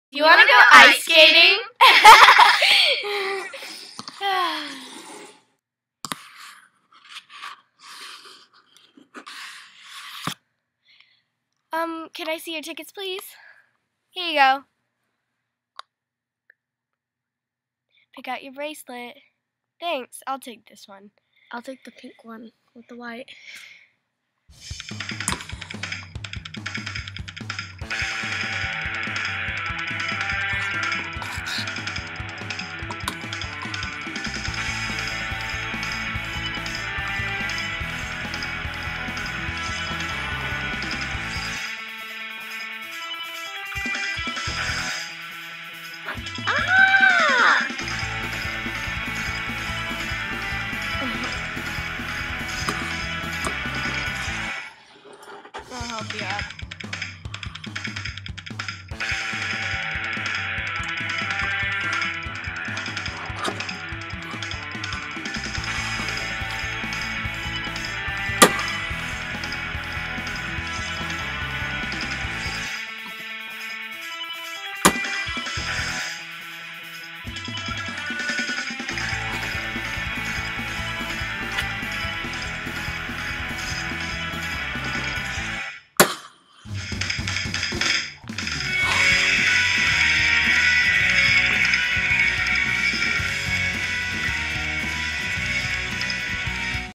ice skates
hey guys! we saw beautiful snow and we couldn't help but not making a stop motion. we actually did the ice skating rink on our pool. it was hard but worth it! hope you enjoy it!